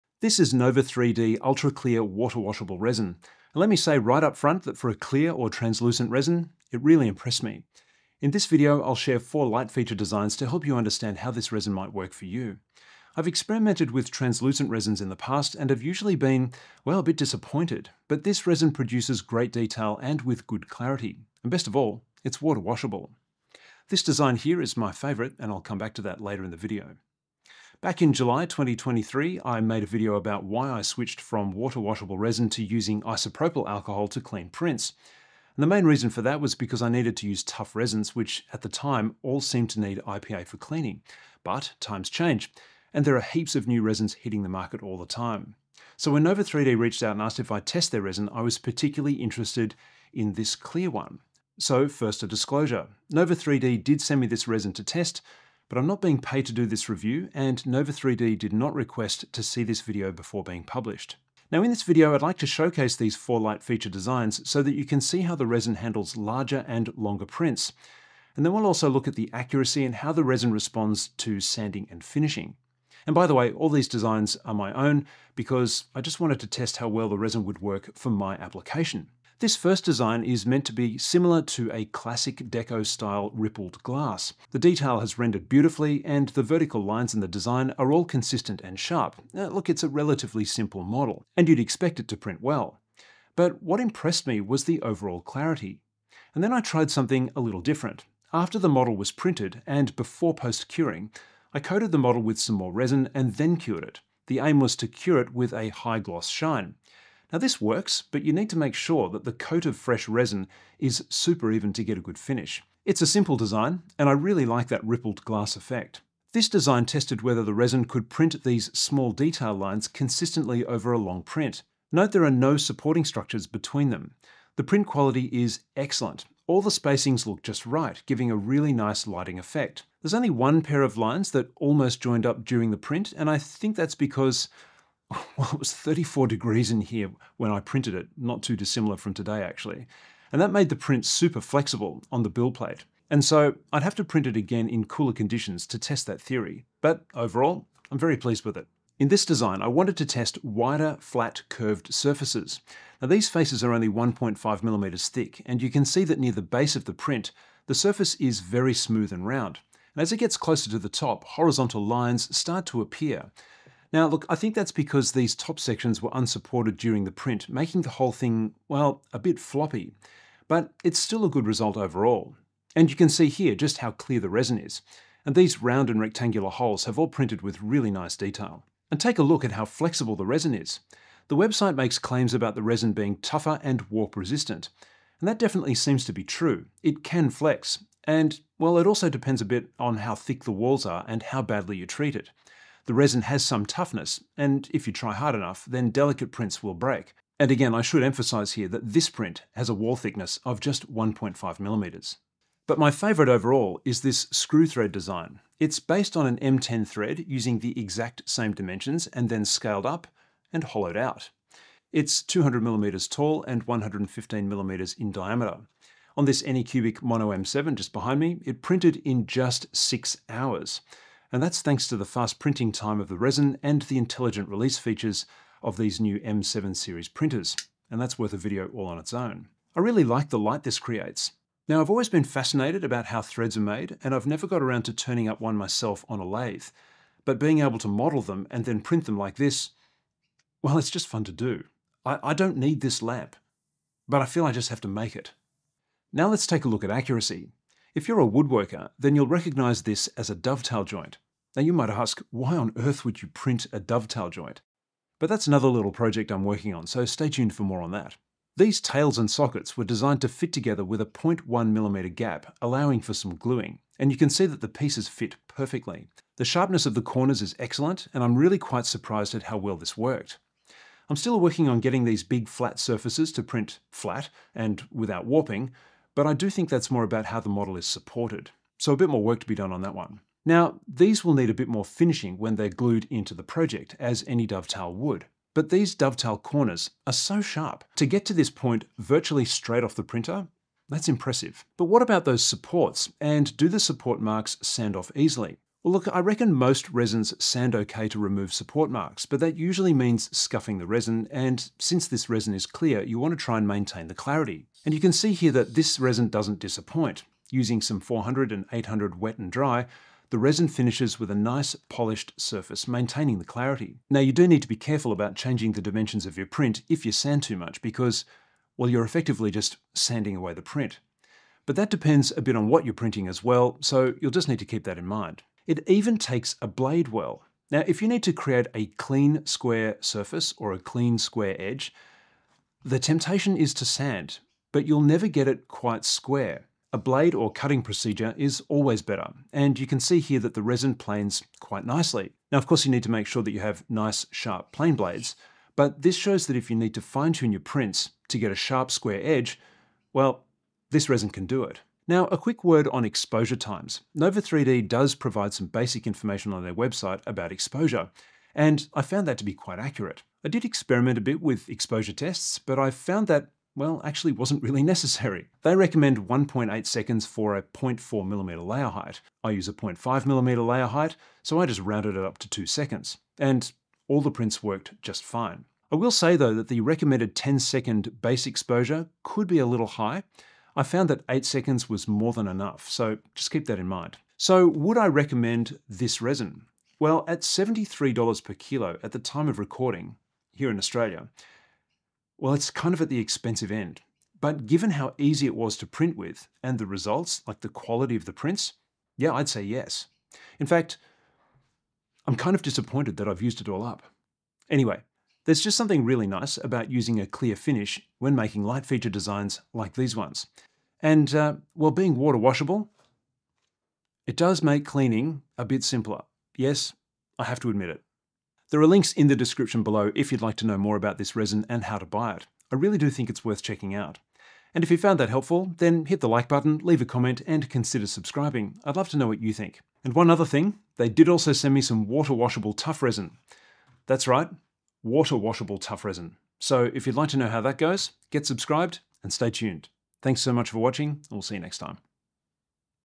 Excellent clear water washable resin - NOVA3D
Nova3D Water Washable Ultra Clear Resin - really worth checking out.

Washable Ultra Clear Resin
Amazon US
Amazon UK
Amazon DE
Amazon IT
Amazon FR

Nova3D Recommended Printing Parameters
Parameters for Water-washable Ultra Clear Resin
Exposure Time: 1.8s
Bottom Exposure Time: 10s
Transition Layer Count: 4
Rest Time: 5s
Lift Height: 6mm